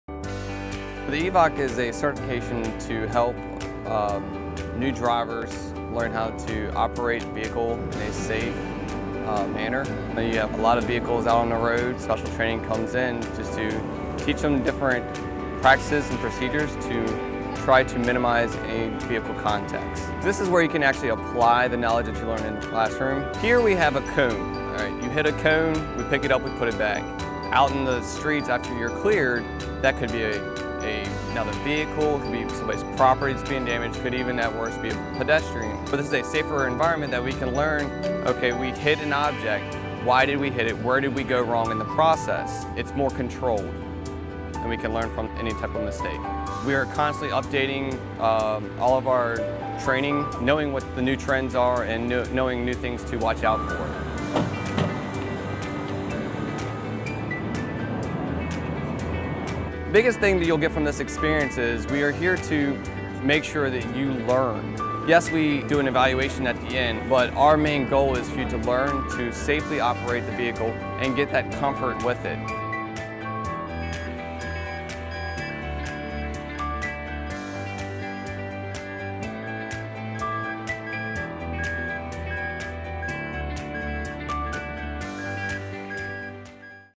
Emergency Vehicle Operators Course
Our new employees go through our award winning Emergency Vehicle Operators Course (EVOC).